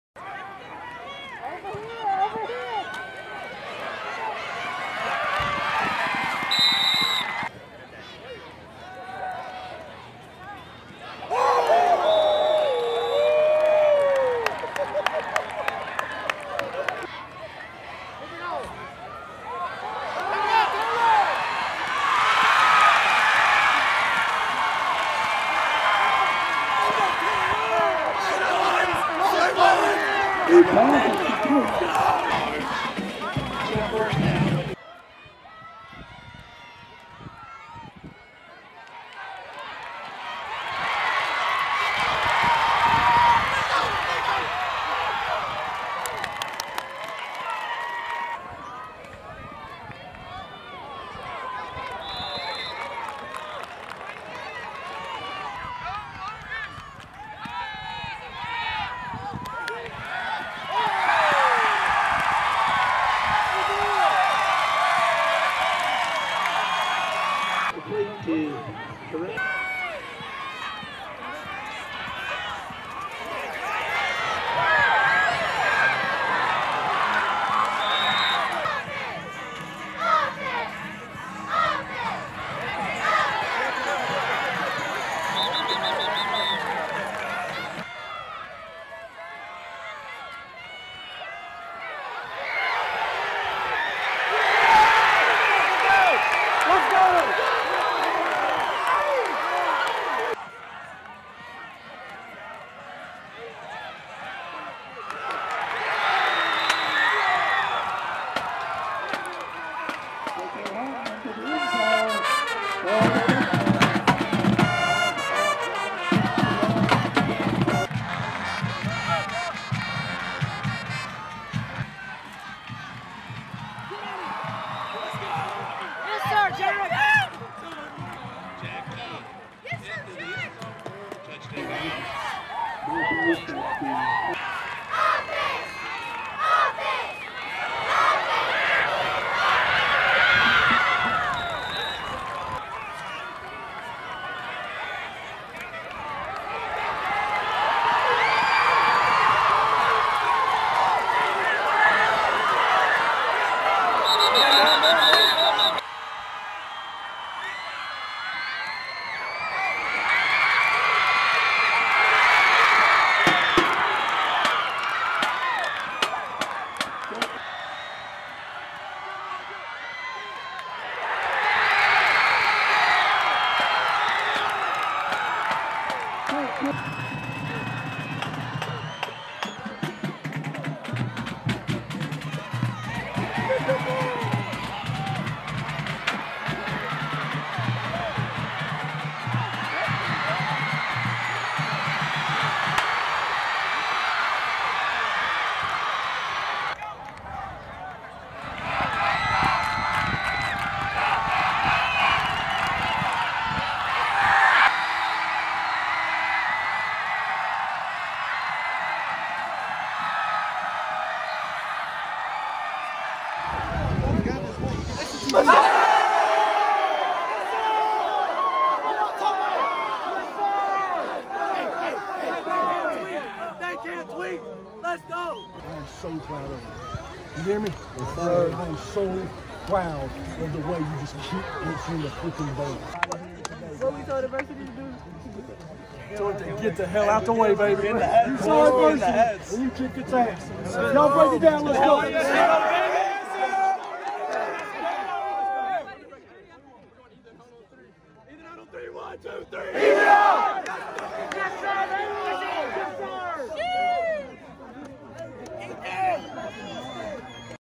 Ocean Springs wins shootout over Biloxi 31-29
A second-half shootout between Biloxi and Ocean Springs brought the game down to the wire. Ocean Springs made a last-second field goal to seal the 31-29 win over the Indians. Here's the action-packed second half highlights.